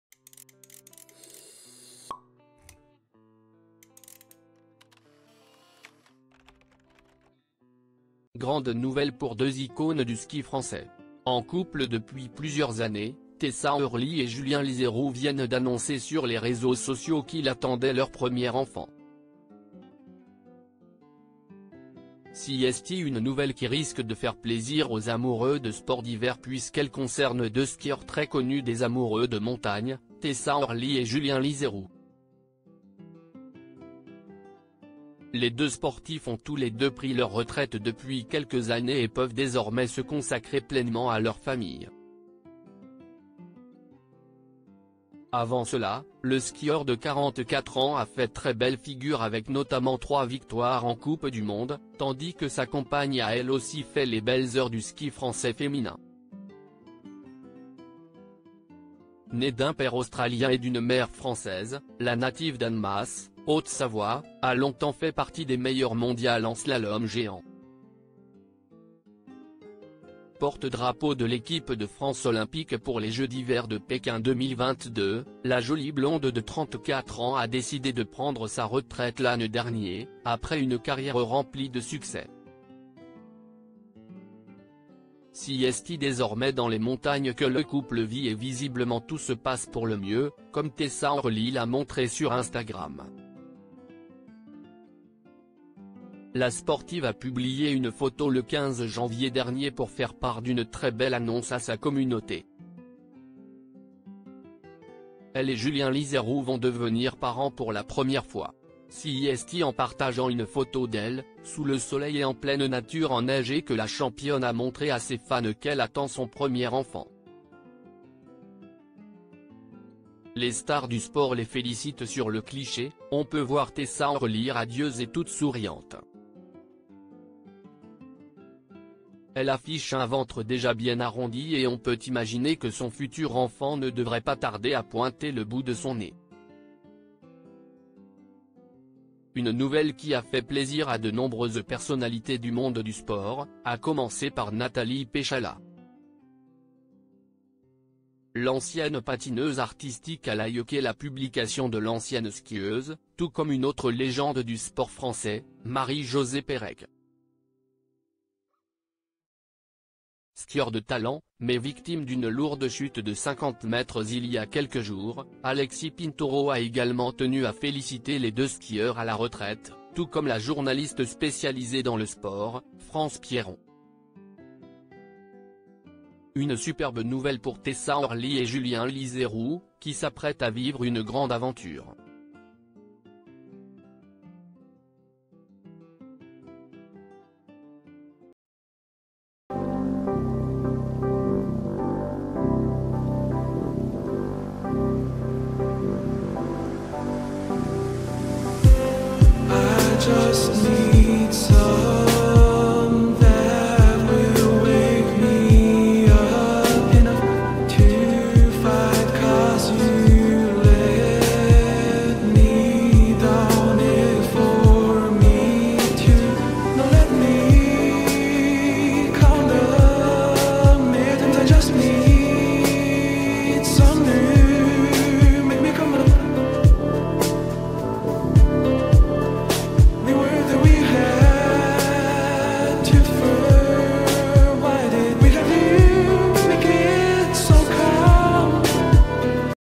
PHOTO Tessa Worley et Julien Lizeroux bientôt parents pour la première fois ! La jolie blonde dévoil
PHOTO Tessa Worley et Julien Lizeroux bientôt parents pour la première fois ! La jolie blonde dévoile son ventre très arrondi